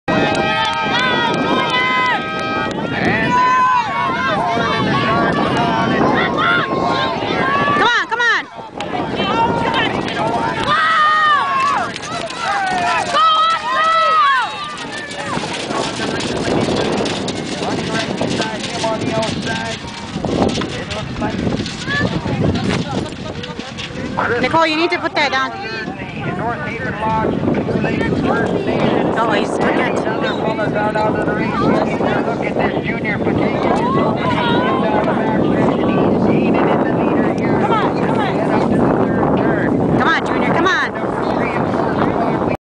Onion lake. cheriot's team pahtayken raceing.MPG
the canada day celibration onion lake cree nation 2007 through 2009